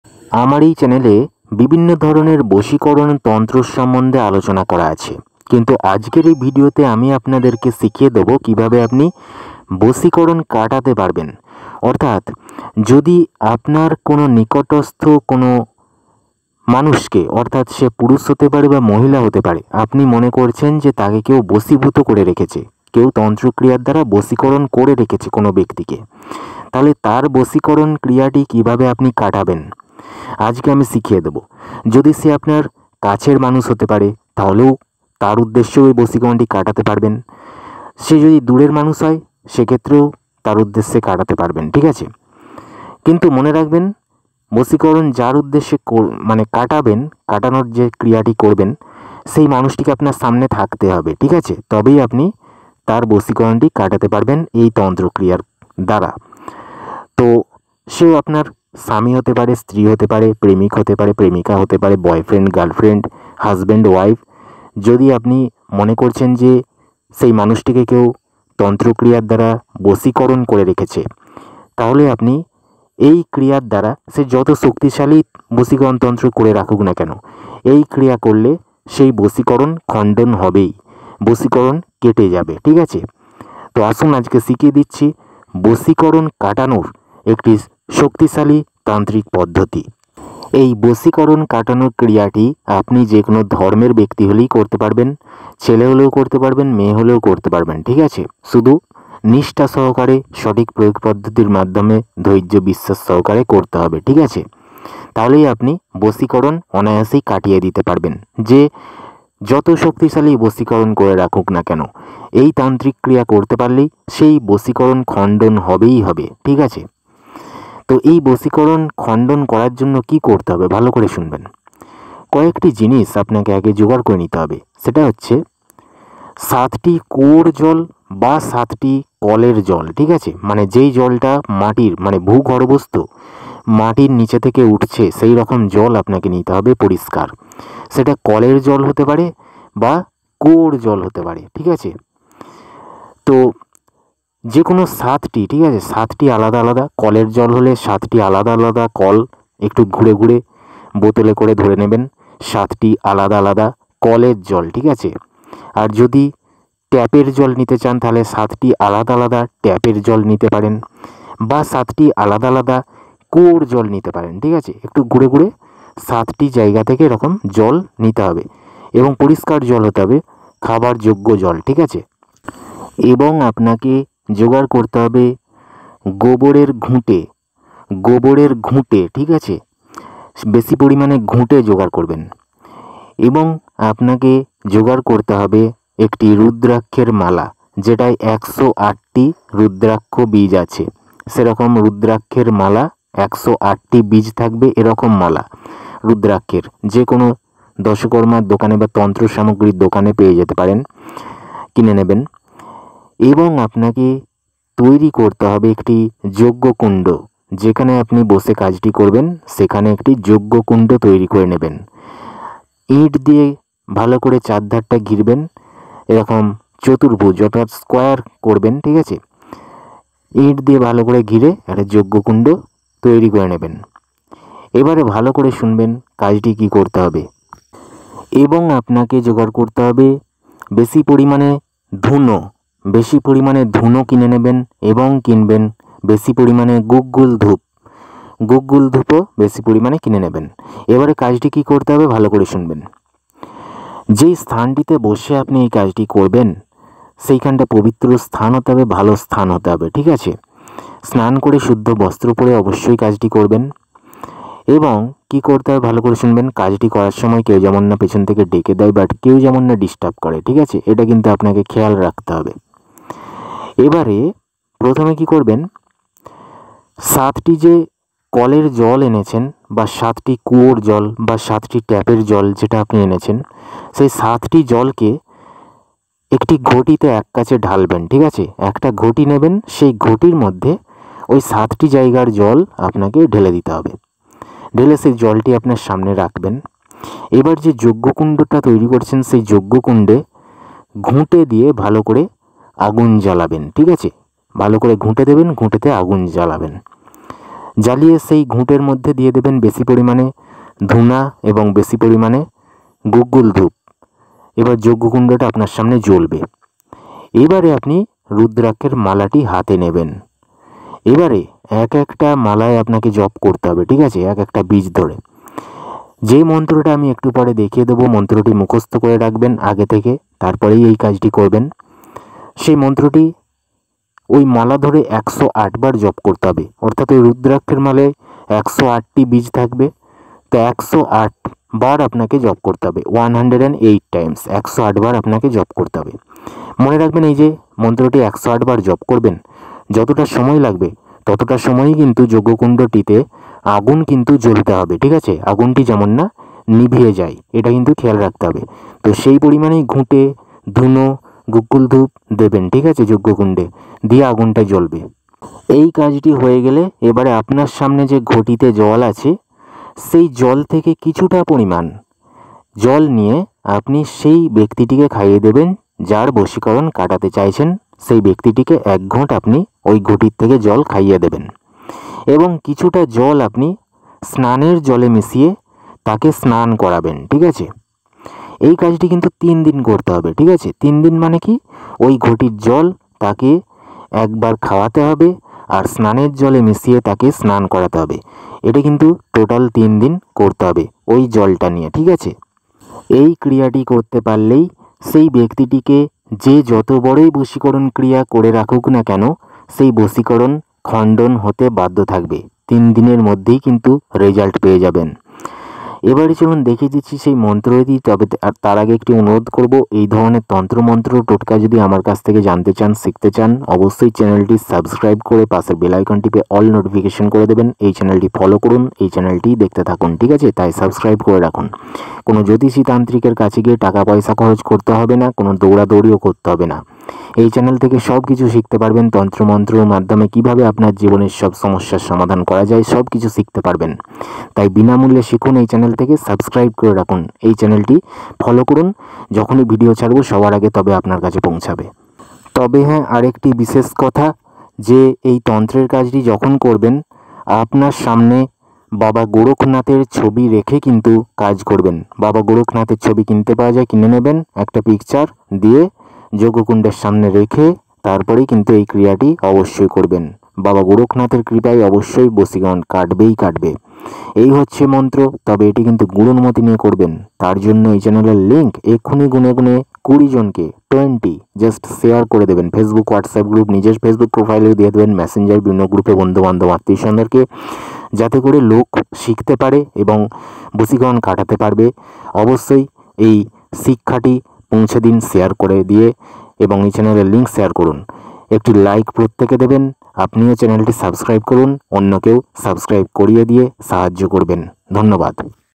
১সেকেন্ডে বশীকরণ কাটানোর সহজ উপায়⚡বশীকরণ থেকে মুক্তির উপায়💥শুধু এই জল খাইয়ে দেবেন এবং স্নান করাবেন
১সেকেন্ডে বশীকরণ কাটানোর সহজ উপায়⚡বশীকরণ থেকে মুক্তির উপায়💥শুধু এই জল খাইয়ে দেবেন এবং স্নান করাবেন